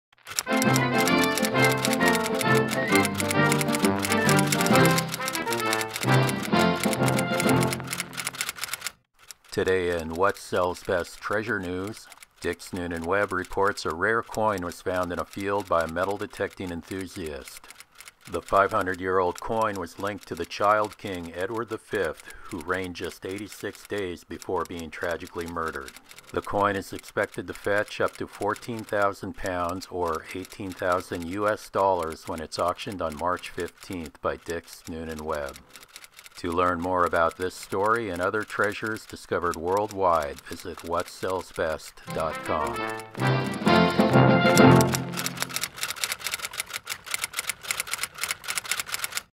FOUND - $18,000 Coin Discovered by Man with Metal Detector
LONDON, UK - Dix Noonan Webb reports a rare 500-year-old coin linked to a child King, has been found by a metal detecting Enthusiast. The coin is expected to fetch up to $18,000 (£15,000) at auction on March 15, 2017.